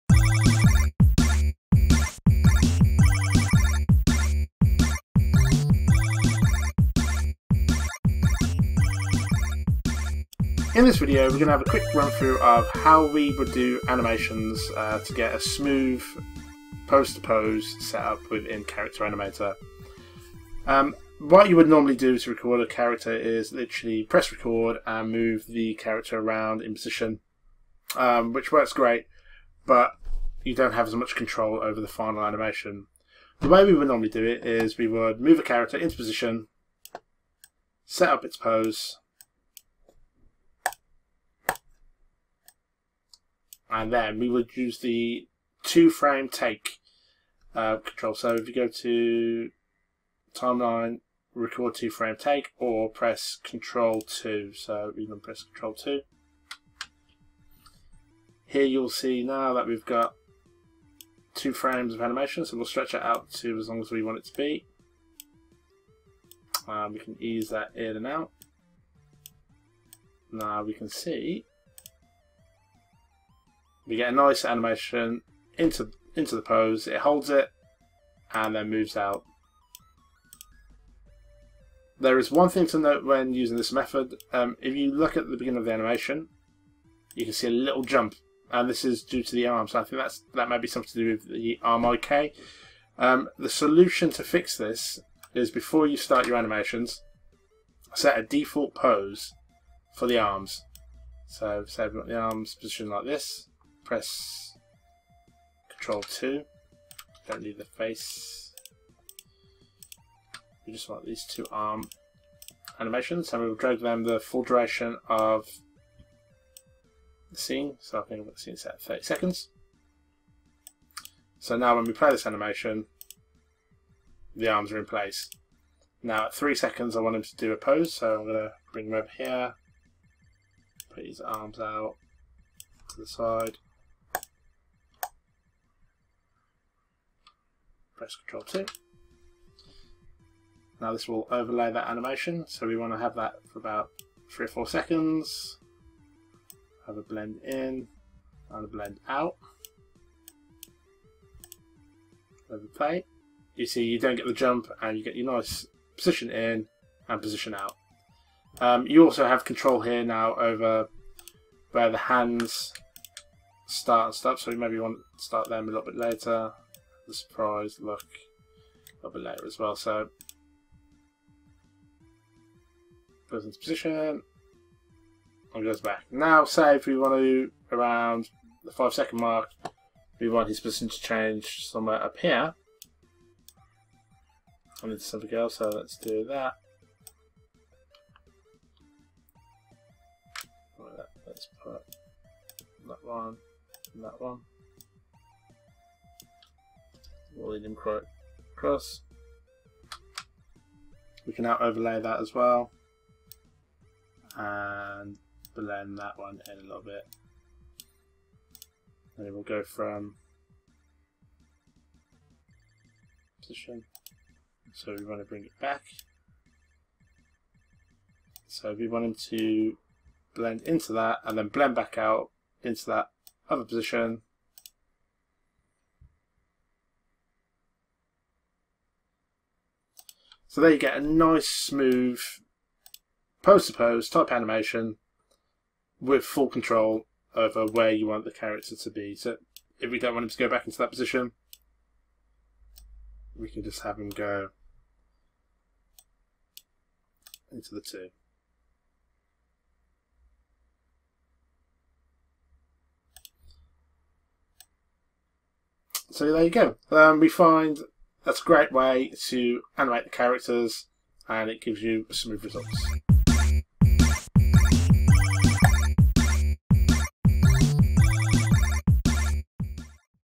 Adobe Character Animator Tutorial: How to do smooth Pose to Pose Animation
Here is more CH tips from Antony Evans, co owner of Digital Puppets. In this video he talks you through how you can create better looking animation using Adobe Character animator Pose to Pose feature